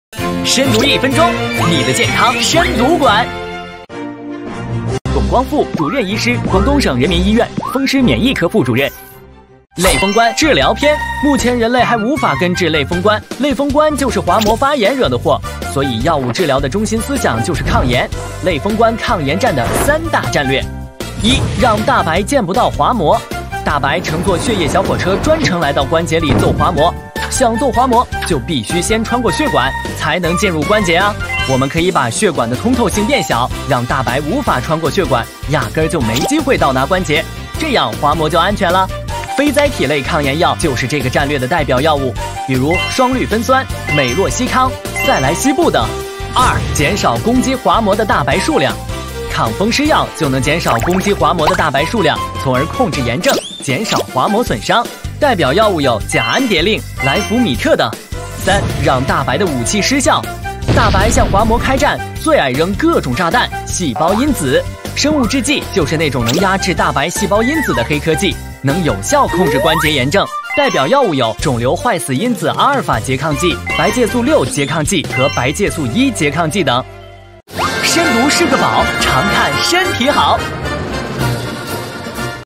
深读小视频-类风湿性关节炎（09）如何治疗类风关？
如何治疗类风关？类风关能根治吗？类风关治疗思路是什么？非甾体类抗炎药有哪些不良反应？长期使用会引起消化道溃疡、出血？抗风湿药能减少大白数量控制炎症？也会引起肝损伤？生物制剂能有效控制关节炎症？